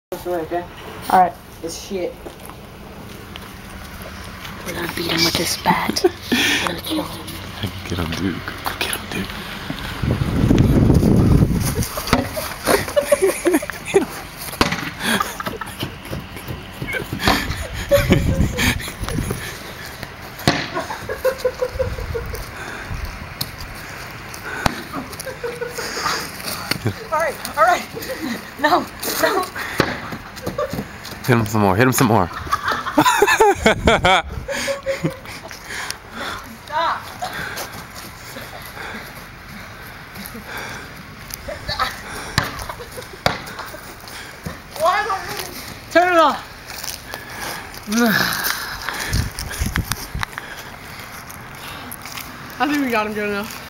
AssJakked - Bat Slap
@$$JaKkeD is doing excatly what "jackass" says not to do...  this is us beating up Cody with a bat.